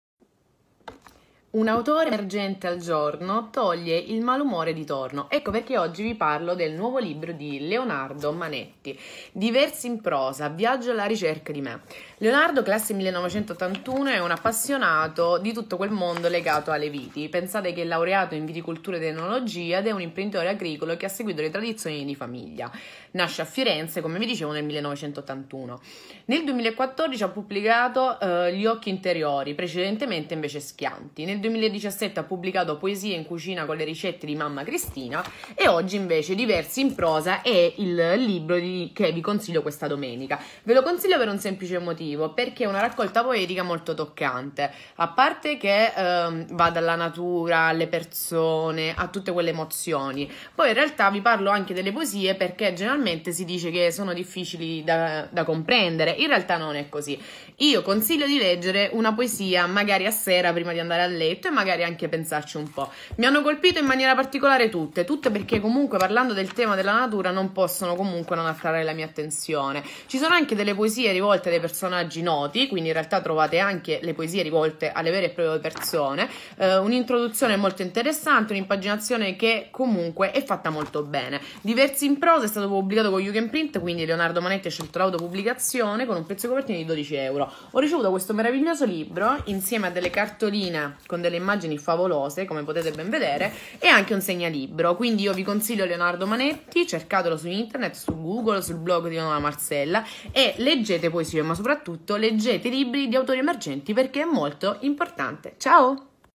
DiVersi in Prosa di Leonardo Manetti
Un viaggio, quello che ci invita a fare Leonardo Manetti, attraverso versi in prosa per la vita, attraverso la natura, alcune fasi dell’esistenza umana.

Un viaggio che tutti, prima o poi, dovremmo compiere.